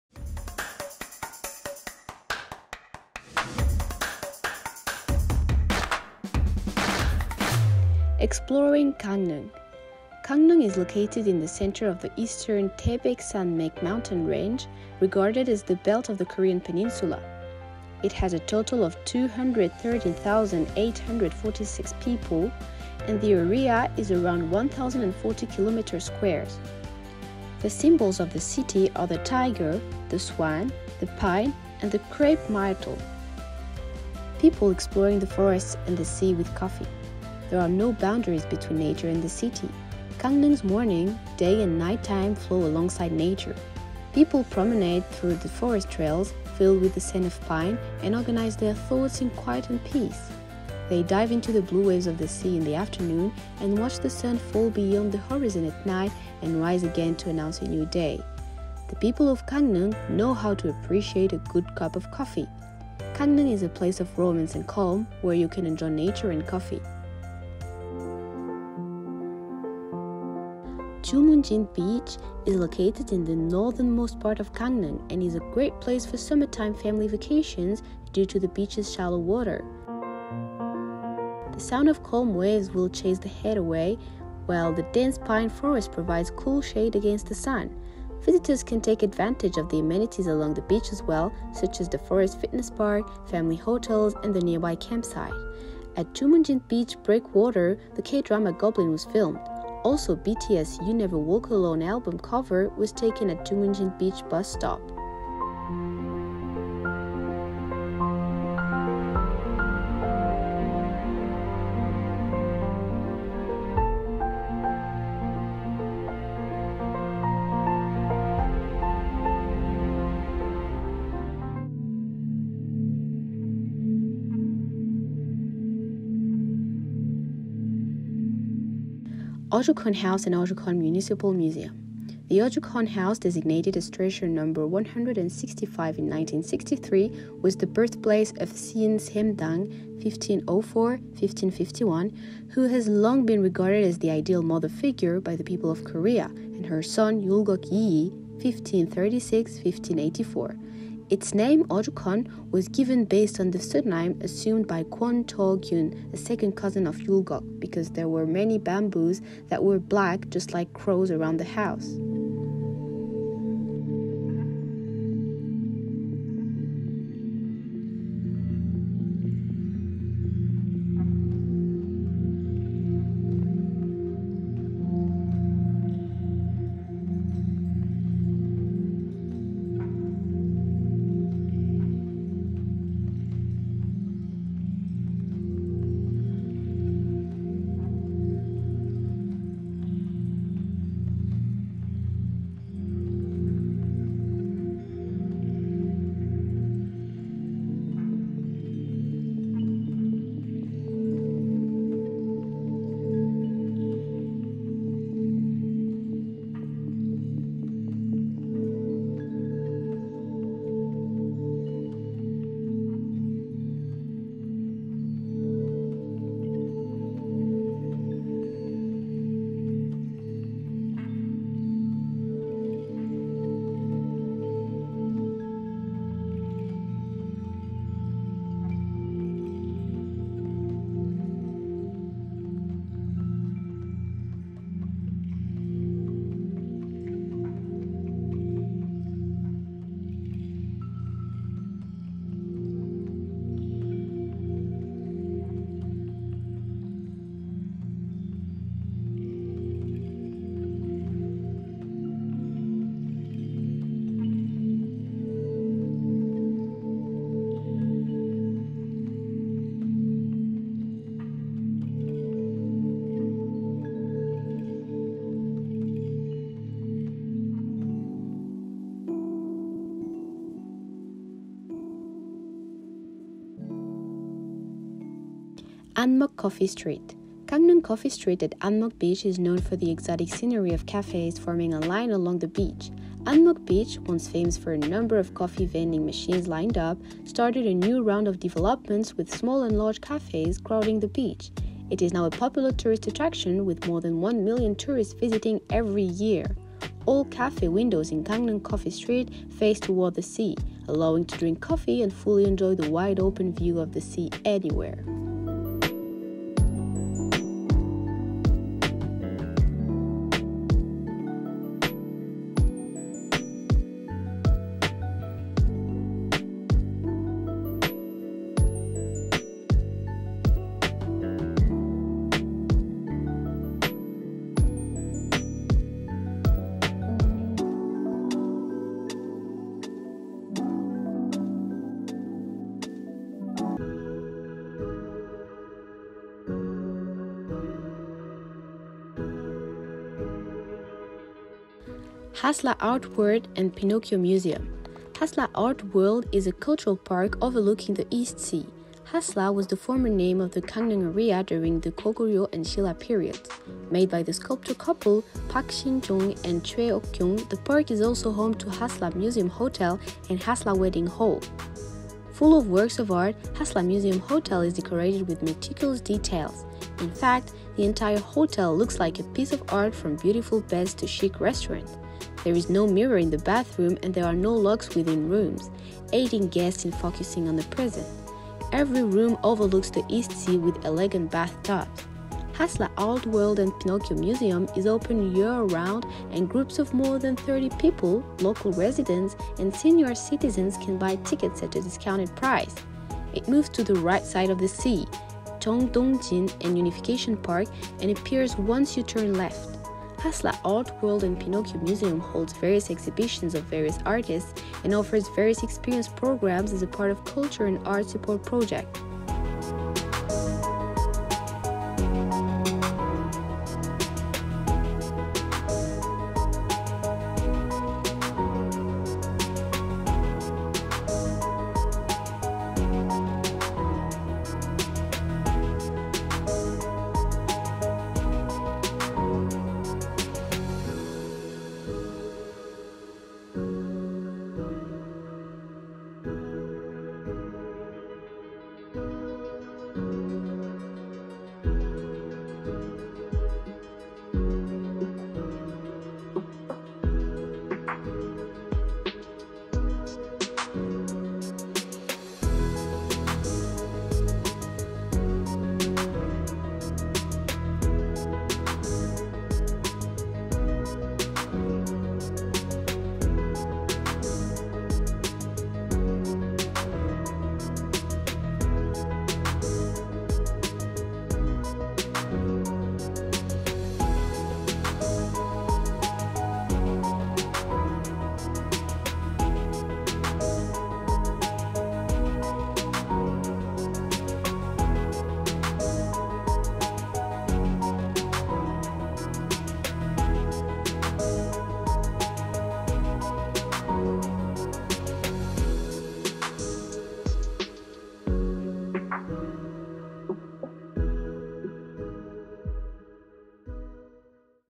Gangneung City Tour in Gangwondo, Korea(외국인이 좋아하는 강릉 관광지)
There are no boundaries between nature and the city.
Gangneung’s morning, day and nighttime flow alongside nature.
People promenade through the forest trails, filled with the scent of pine, and organize their thoughts in quiet and peace.
They dive into the blue waves of the sea in the afternoon, and watch the sun fall beyond the horizon at night and rise again to announce a new day.
The people of Gangneung know how to appreciate a good cup of coffee.
Gangneung is a place of romance and calm, where you can enjoy nature and coffee.